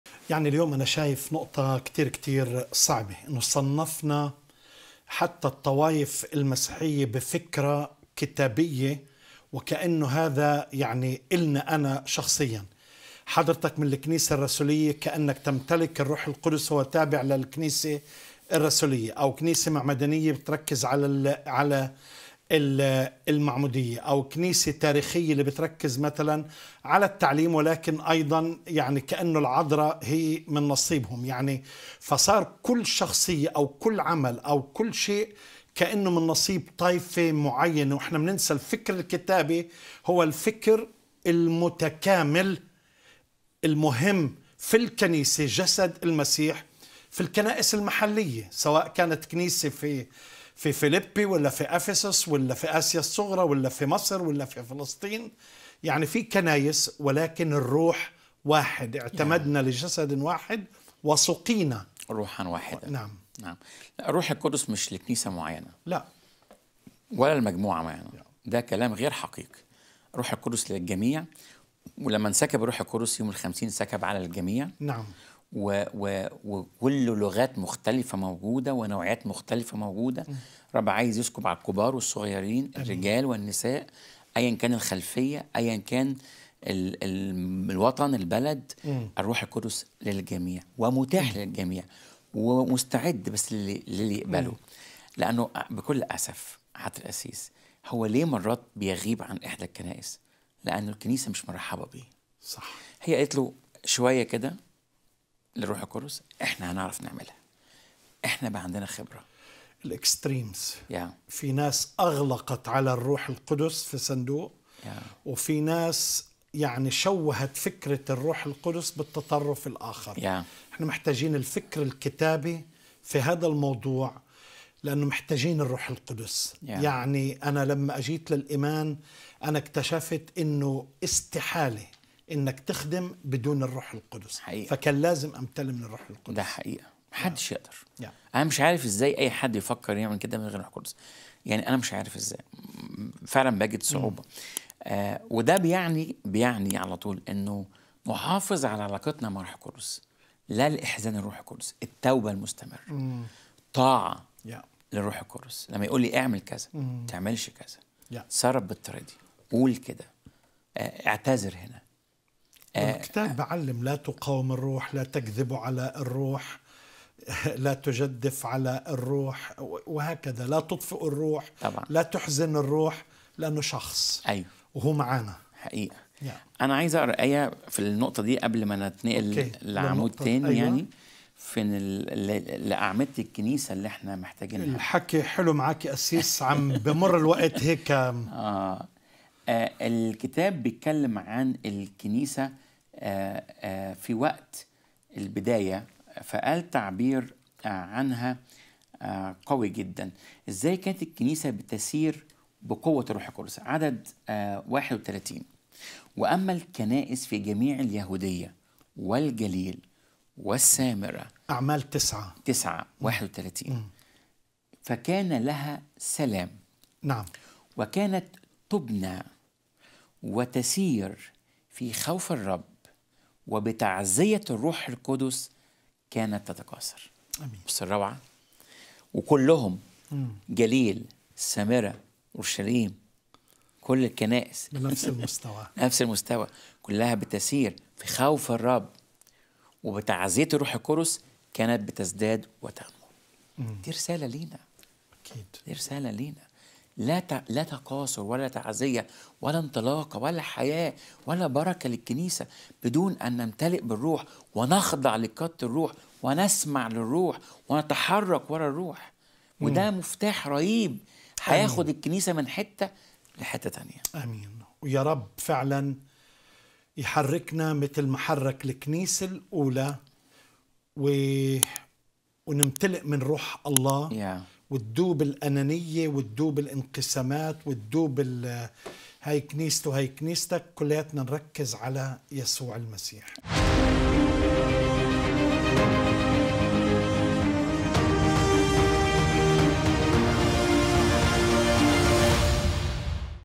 الروح القدس ليس حكراً على كنيسة أو طائفة معينة
"روح واحد، جسد واحد: هل نفهم حقًا معنى الوحدة في المسيح؟
تجاوز الانقسامات: الروح القدس للجميع بلا استثناء

🔸وحدة الروح القدس: تأكيد على أن الروح القدس هو للجميع وليس حكرًا على طائفة أو كنيسة معينة.
🔸أهمية ملء الروح القدس: ضرورة ملء الروح القدس في حياة المؤمنين لخدمة الله بكفاءة وفعالية.
🔸دور الروح القدس في بناء الكنيسة: دور الروح القدس في توحيد المؤمنين وبناء كنيسة قوية وناشطة.
🔸خطورة تقسيم الروح القدس: تحذير من خطورة تقسيم الروح القدس وتخصيصه لطائفة معينة.
🔸أهمية الخضوع للروح القدس: ضرورة الخضوع لقيادة الروح القدس في الحياة اليومية.

📌يمكنك التواصل معنا بصورة شخصية عن طريق رسائل الواتساب👇
WhatsApp الواتساب:    🆆🅷🅰🆃🆂🅰🅿🅿